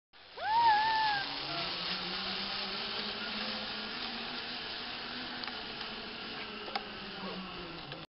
ziplining at Wacky Rollers
just a little video clip that my friend Lucas took of me on the zipline. 6/20/10